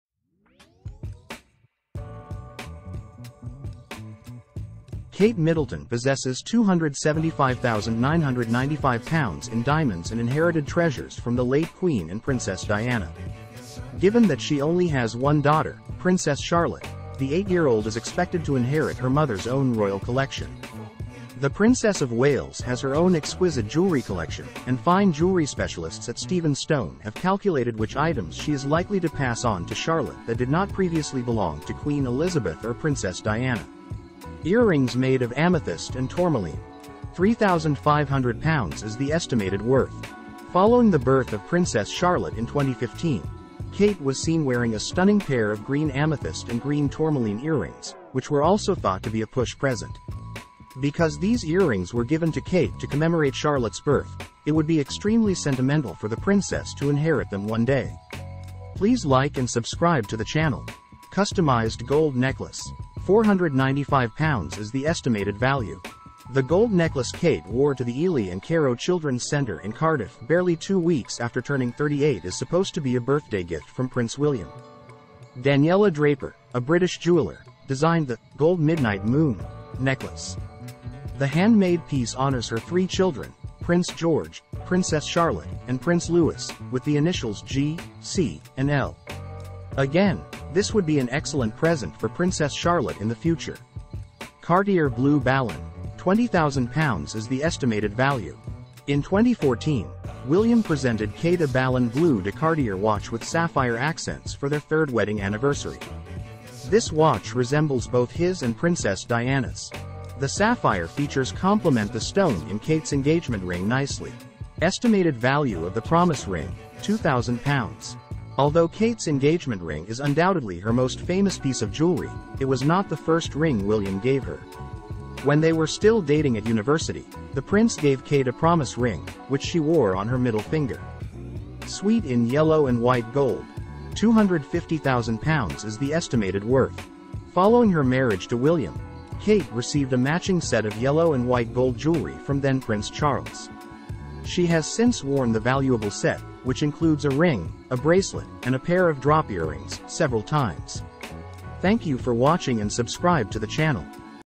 Royal family! Kate Middleton left Charlotte £275,995 worth of jewelry
Kate Middleton possesses £275,995 in diamonds and inherited treasures from the late Queen and Princess Diana. Given that she only has one daughter, Princess Charlotte, the eight-year-old is expected to inherit her mother's own royal collection.
The Princess of Wales has her own exquisite jewelry collection, and fine jewelry specialists at Steven Stone have calculated which items she is likely to pass on to Charlotte that did not previously belong to Queen Elizabeth or Princess Diana.
Earrings made of amethyst and tourmaline
£3,500 is the estimated worth.
Following the birth of Princess Charlotte in 2015, Kate was seen wearing a stunning pair of green amethyst and green tourmaline earrings, which were also thought to be a push present.
Because these earrings were given to Kate to commemorate Charlotte's birth, it would be extremely sentimental for the Princess to inherit them one day.
Customized gold necklace
£495 is the estimated value.
The gold necklace Kate wore to the Ely and Careau Children's Centre in Cardiff barely two weeks after turning 38 is supposed to be a birthday gift from Prince William.
Daniella Draper, a British jeweller, designed the "Gold Midnight Moon" necklace.
The handmade piece honors her three children, Prince George, Princess Charlotte, and Prince Louis, with the initials G, C, and L. Again, this would be an excellent present for Princess Charlotte in the future.
Cartier Blue Ballon
£20,000 is the estimated value.
In 2014, William presented Kate a Ballon Bleu de Cartier watch with sapphire accents for their third wedding anniversary.
This watch resembles both his and Princess Diana's. The sapphire features compliment the stone in Kate's engagement ring nicely.
Estimated value of the promise ring: £2,000
Although Kate's engagement ring is undoubtedly her most famous piece of jewelry, it was not the first ring William gave her.
When they were still dating at university, the Prince gave Kate a promise ring, which she wore on her middle finger.
Suite in yellow and white gold
£250,000 is the estimated worth.
Following her marriage to William, Kate received a matching set of yellow and white gold jewelry from then-Prince Charles.
She has since worn the valuable set, which includes a ring, a bracelet, and a pair of drop earrings, several times.